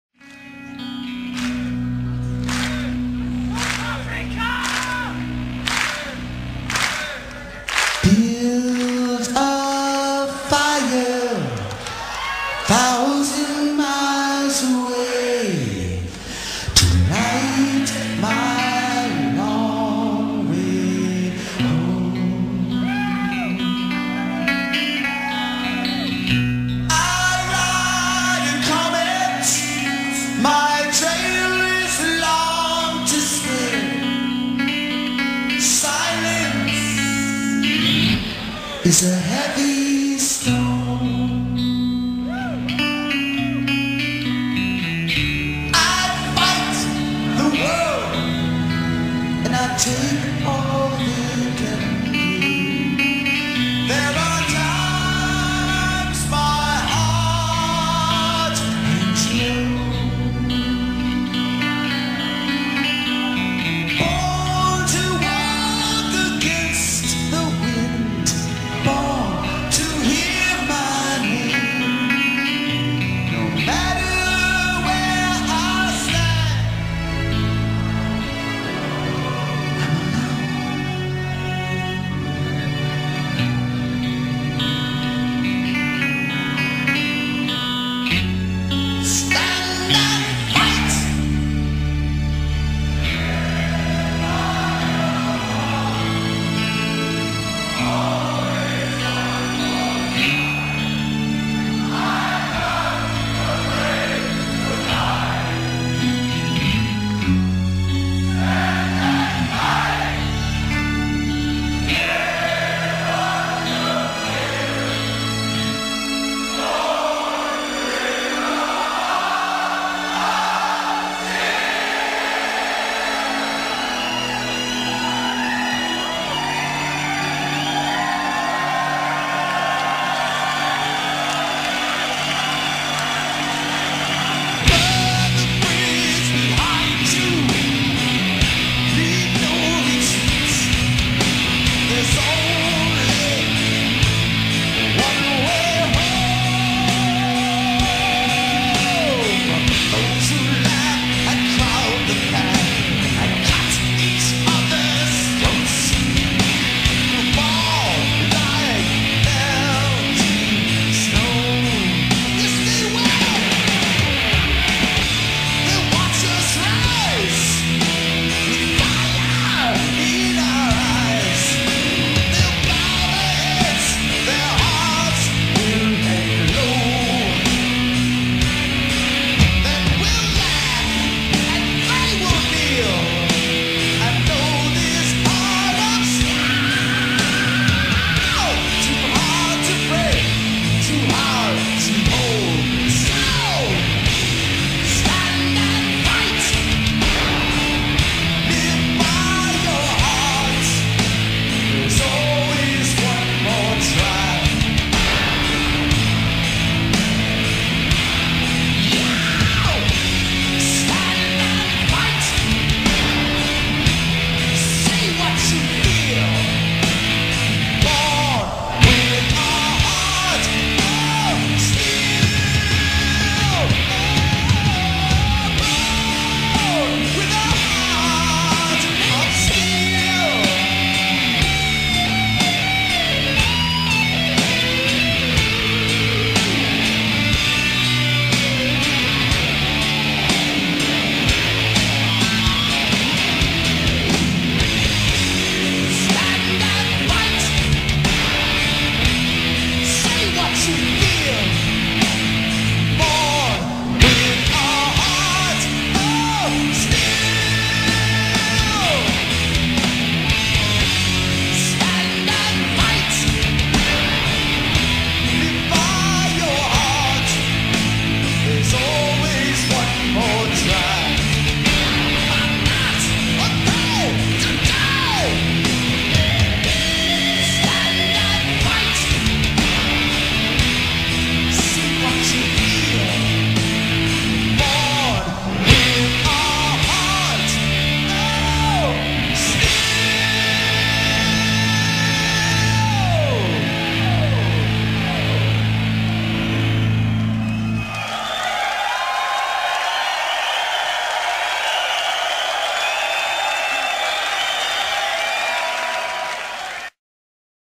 Wrestling Tribute
A wrestling tribute to the following wrestlers:  Mr. Perfect, Kerry Von Erich, Hawk, Big Boss Man, Brian Pillman and more...video quality poor due to restricted youtube and wmg audio rules and having to edit video too many times lol